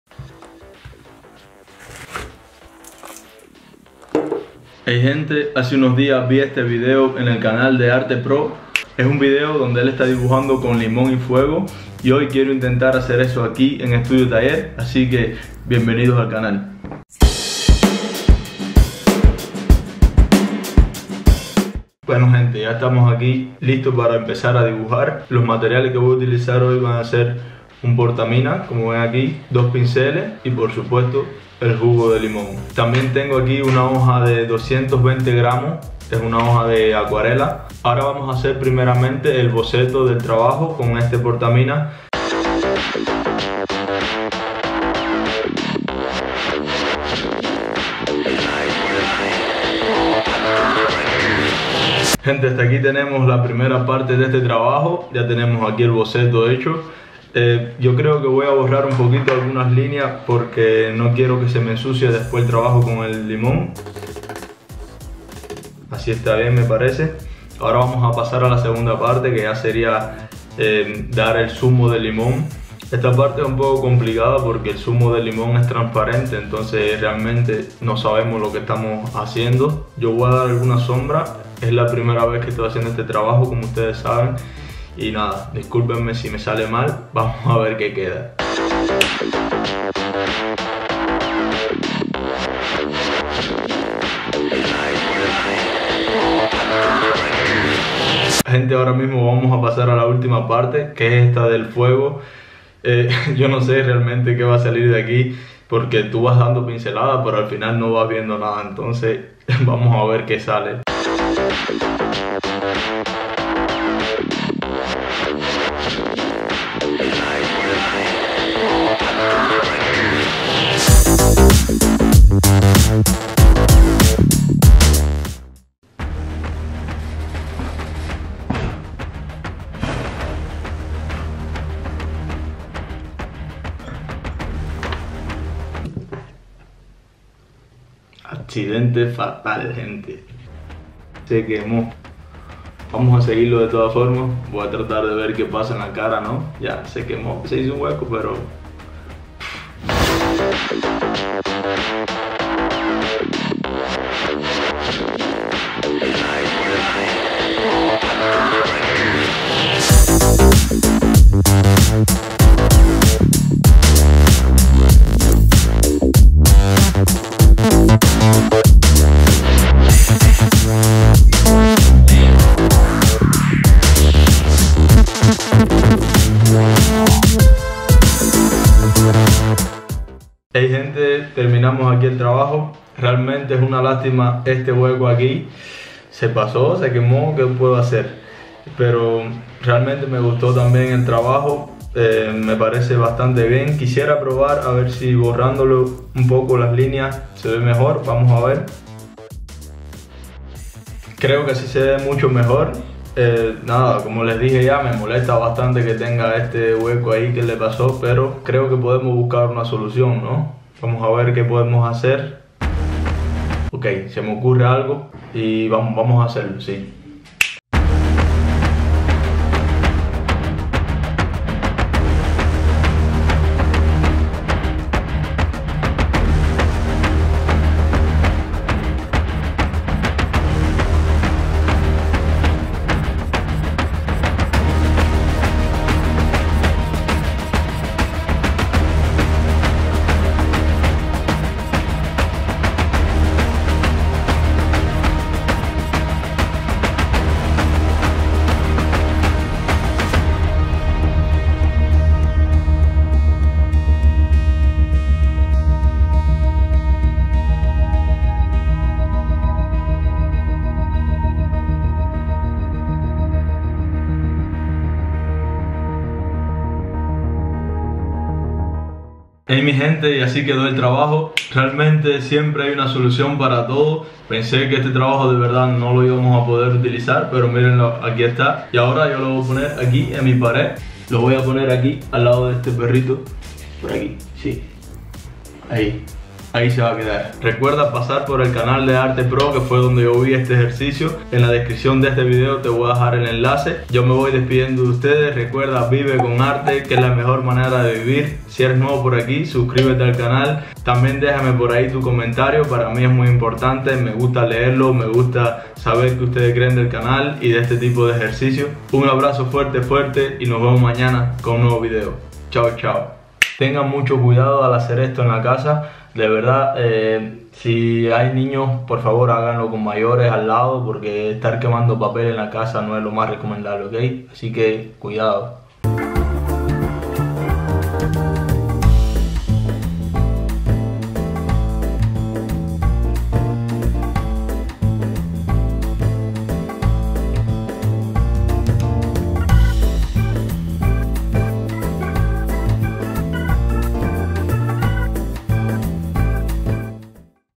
Se me quema un dibujo | Estudio Taller
Dibujando con fuego y limón, un reto muy interesante que vi en el canal de Arte Pro.

Materiales que utilizo.
Carboncillo Faber-Castell
Coates - 20 carboncillos de sauce
Goma moldeable
Lápices carboncillo

Cámara
Canon G7x